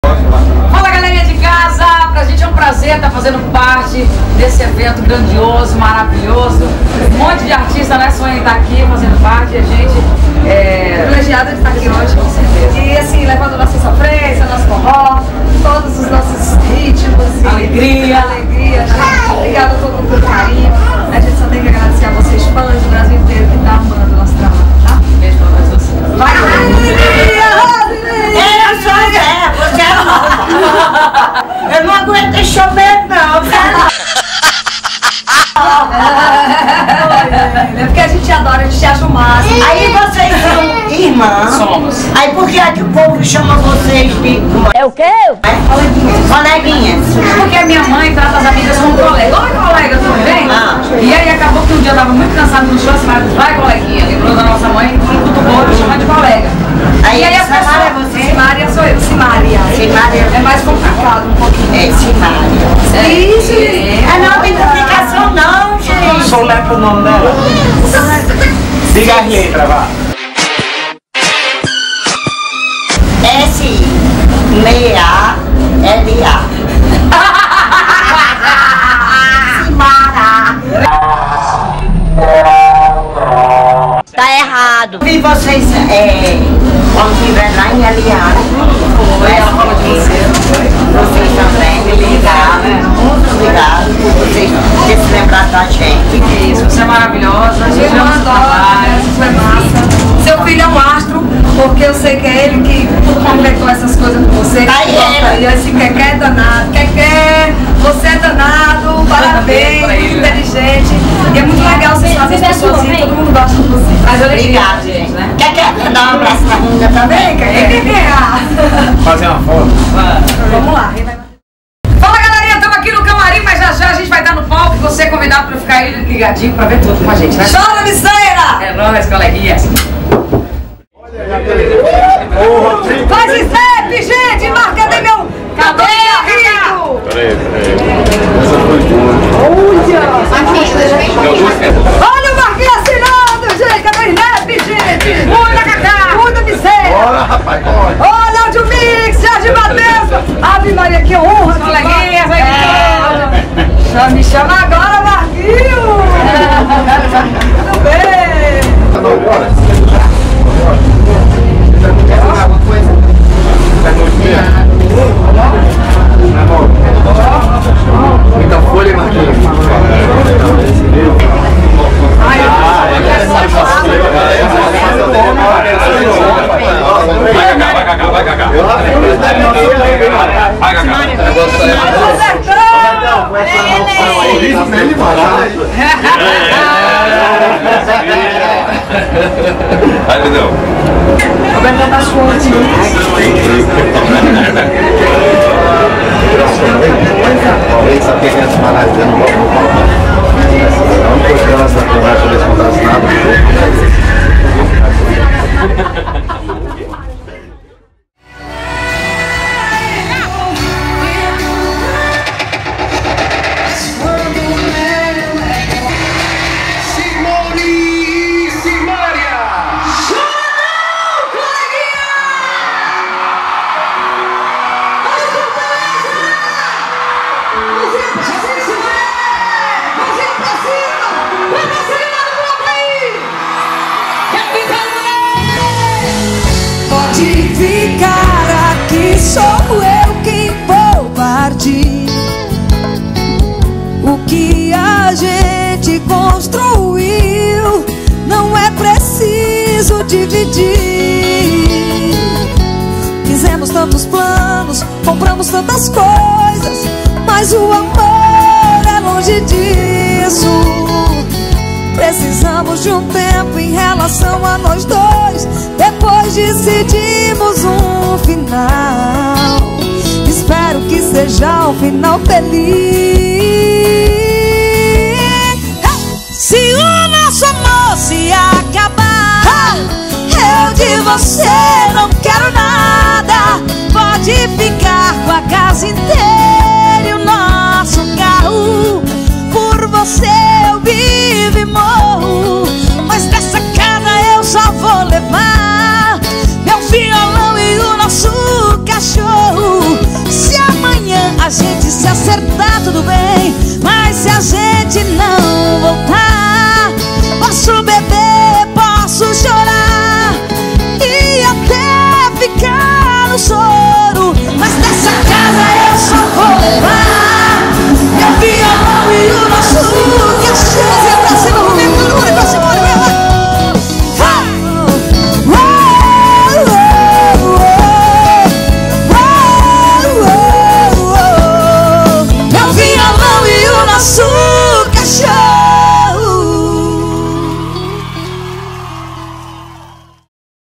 Simone e Simaria Na Estrada #1 Janeiro//2016
Compacto com alguns momentos dos nossos shows entre Dezembro de 2015 a Janeiro de 2016. Obrigada a todos os nossos fã-clubes pelo carinho e respeito de sempre, e a turma do Keké Isso na TV (Dona Irene e Kerginaldo), vocês são ÓTIMOS!

Imagens/Edição: Heber Barros